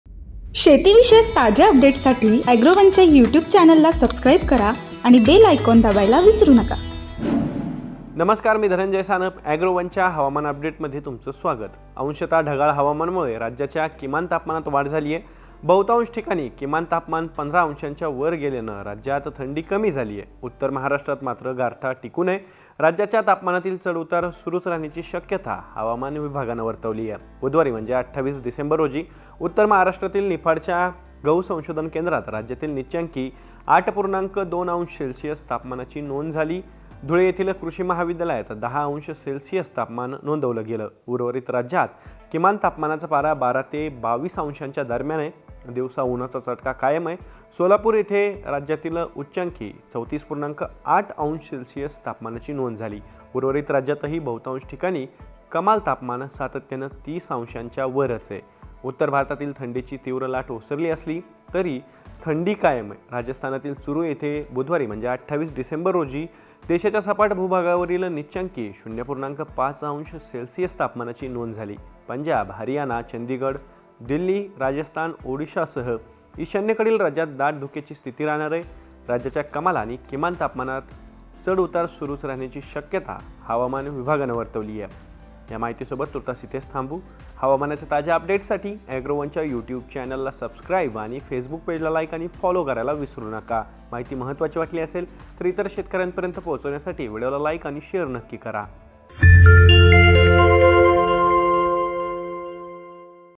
Weather Update : Fluctuations in temperature will continue in the state| Agrowon
बुधवारी (ता. २८) उत्तर महाराष्ट्रातील निफाडच्या गहू संशोधन केंद्रात राज्यातील नीचांकी ८.२ अंश सेल्सिअस तापमानाची नोंद झाली आहे. धुळे येथील कृषी महाविद्यालयात १० अंश सेल्सिअस तापमान नोंदले गेले. उर्वरित राज्यात किमान तापमानाचा पारा १२ ते २२ अंशांच्या दरम्यान आहे.